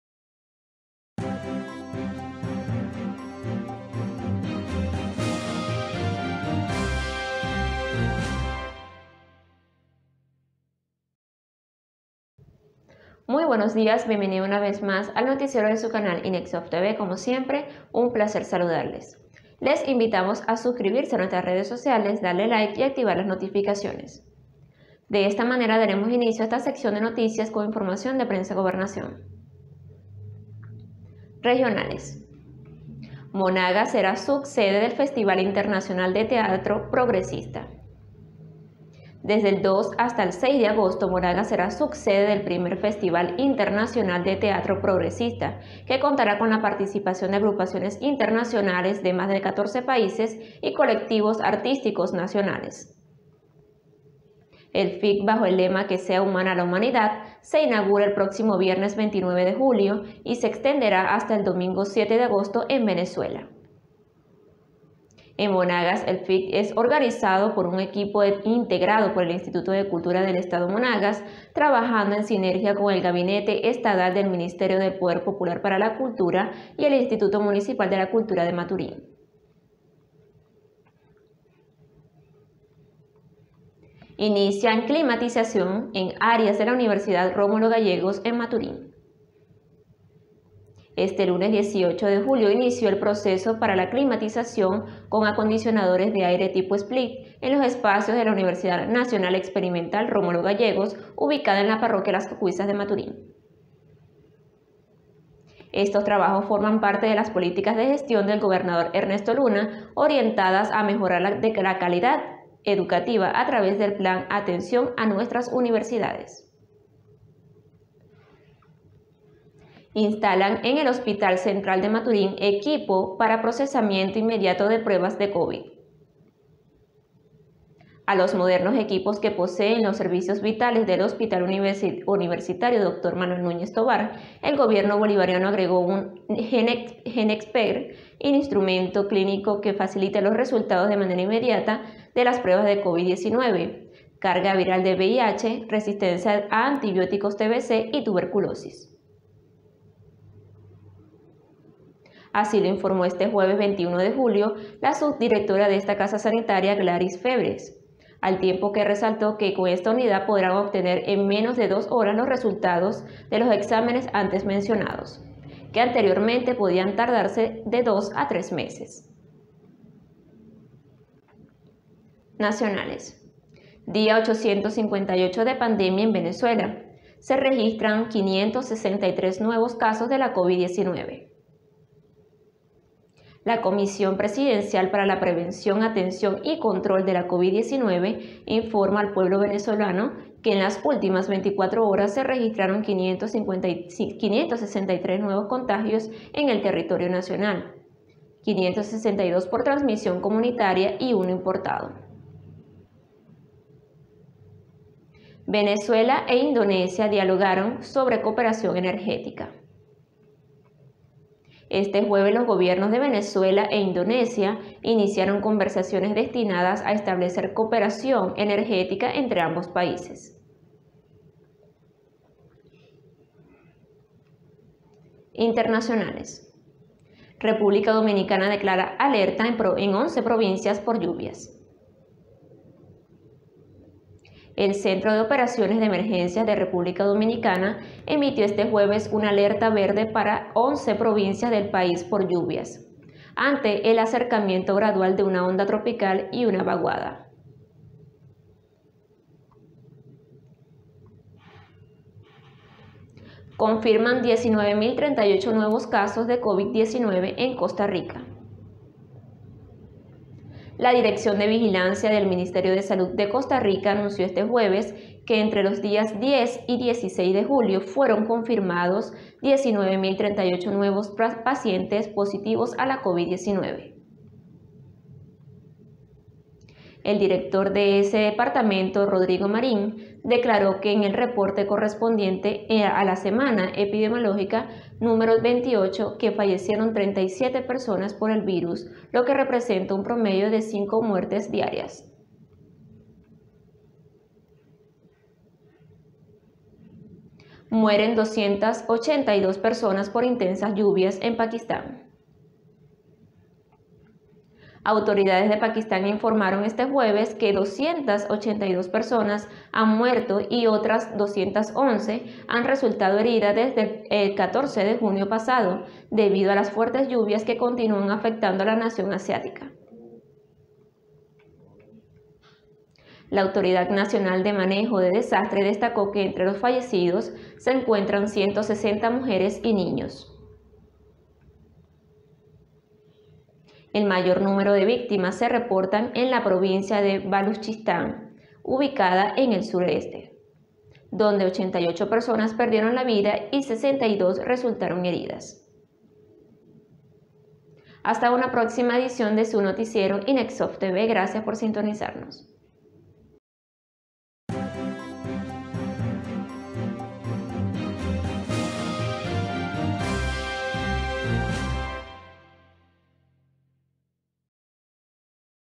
🔴 Confirman 19038 nuevos casos de Covid19 en CostaRica
REGIONALES

MONAGAS SERÁ SUB SEDE DEL FESTIVAL INTERNACIONAL DE TEATRO PROGRESISTA

INICIAN CLIMATIZACIÓN EN ÁREAS DE LA UNIVERSIDAD RÓMULO GALLEGOS EN MATURÍN

INSTALAN EN EL HOSPITAL CENTRAL DE MATURIN EQUIPO PARA PROCESAMIENTO INMEDIATO DE PRUEBAS COVID

NACIONALES

DÍA 858 DE PANDEMIA EN VENEZUELA: SE REGISTRAN 563 NUEVOS CASOS DE LA COVID-19

VENEZUELA E INDONESIA DIALOGARON SOBRE COOPERACIÓN ENERGÉTICA

INTERNACIONALES

REPÚBLICA DOMINICANA DECLARA ALERTA EN 11 PROVINCIAS POR LLUVIAS

CONFIRMAN 19.038 NUEVOS CASOS DE COVID-19 EN COSTA RICA

MUEREN 282 PERSONAS POR INTENSAS LLUVIAS EN PAKISTÁN